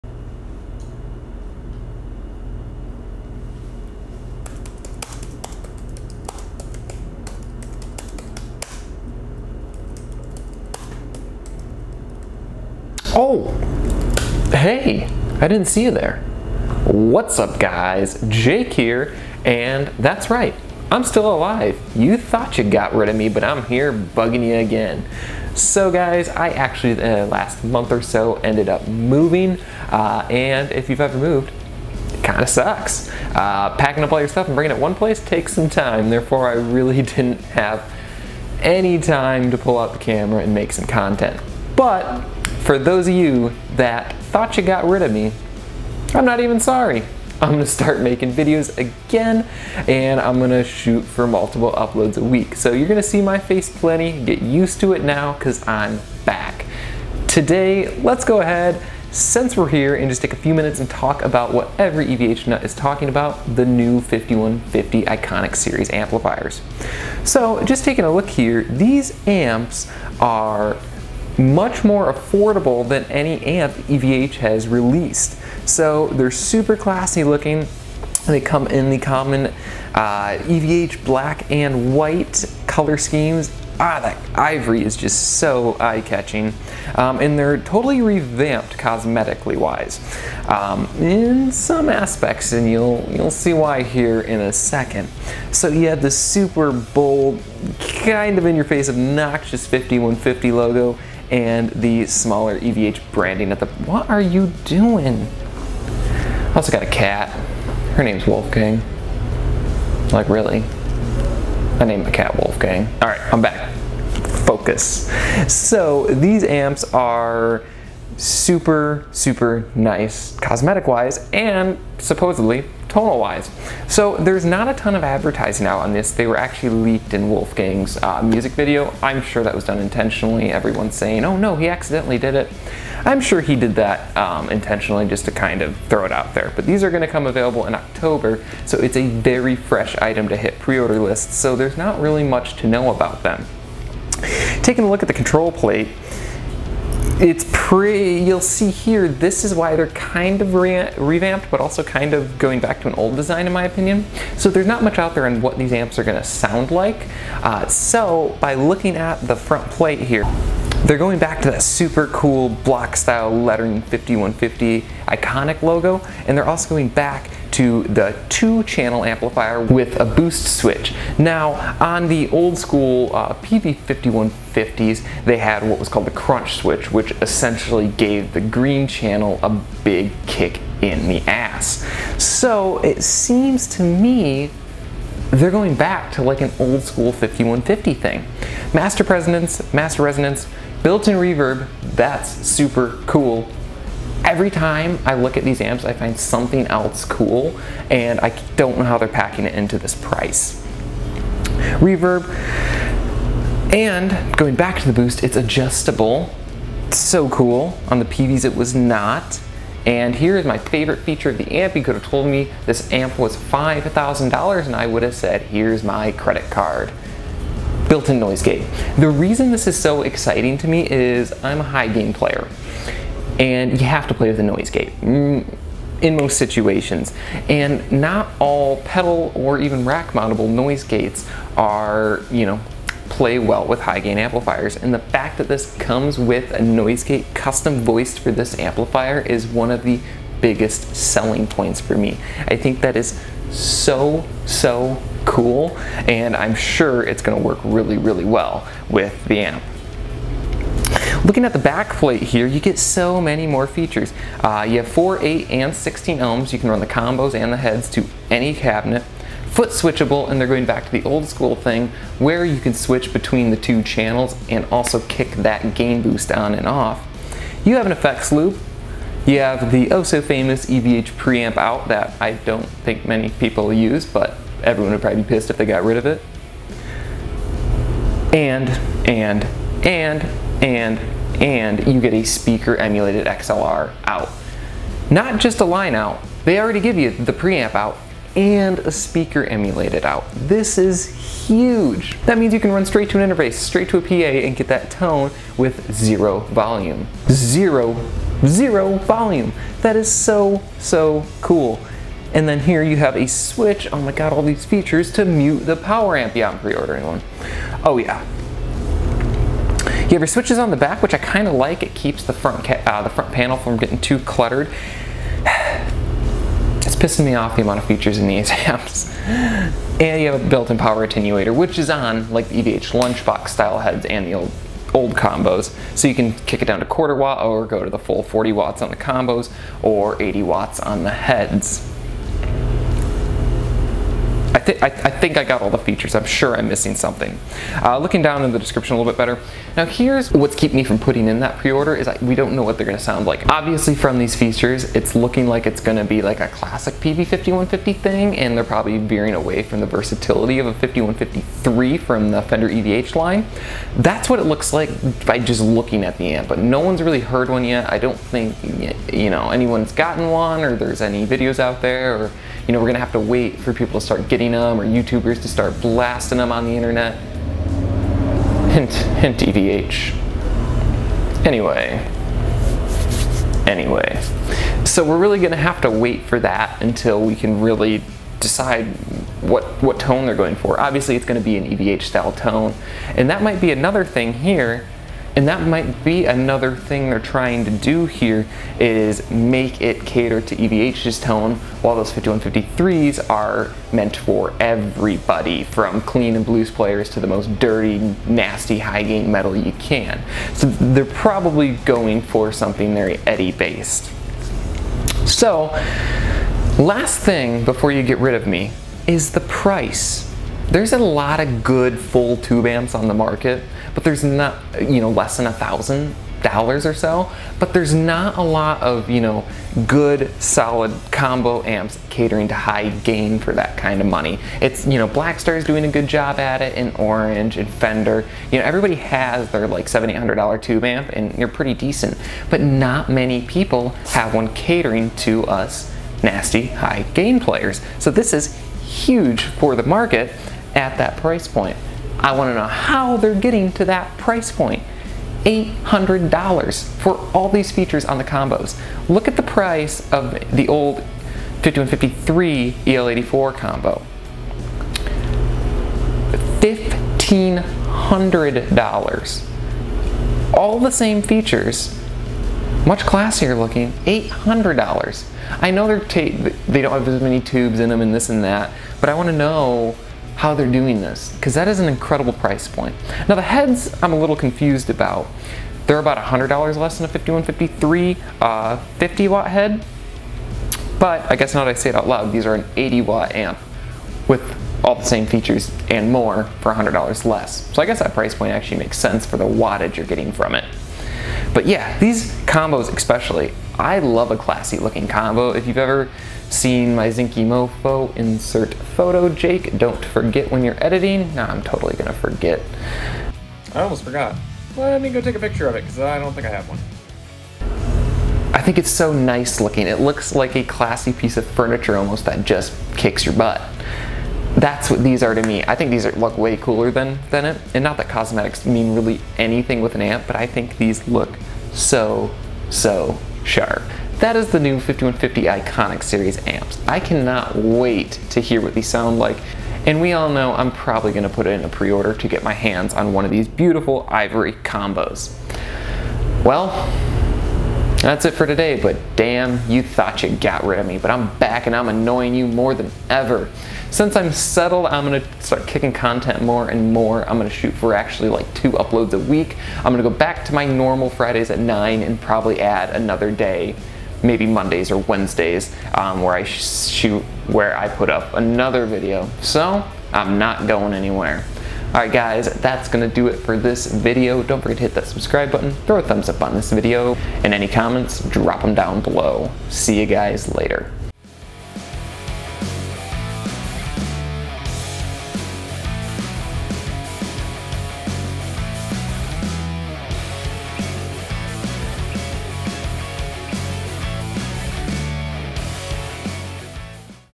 EVH 5150 IV!? | New EVH 5150 Iconic Series Amp | I'm Back!
After years of waiting, we finally have a new 5150 from the EVH Fender line to look forward to. The 5150 Iconic series will come in a 40 watt combo, and also a 80 watt head. These are catering to a affordable price, but also feature packed. Attenuator, emulated out, effects loop, build in noise gate, a gain boost, and so much more. I am so excited to get my hands on one of these, as I'm sure they will sound great!

What do you think of this new release? Let me know down below in the comments!

Jake Tanis is an endorsee of Sfarzo Guitar Strings!
sfarzo.us